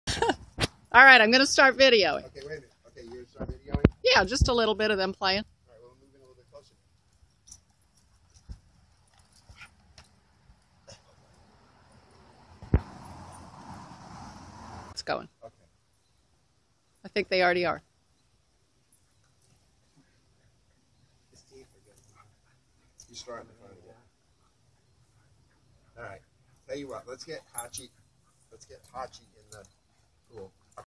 Playmates Hachi and Kodi wrestling and play fighting..
Kodi and his classmate/playmate Hachi (with her owner Tom) in our backyard playing like Huskies do.  :-)  Hachi pins Kodi in her "Ladies Rule" move.  :-)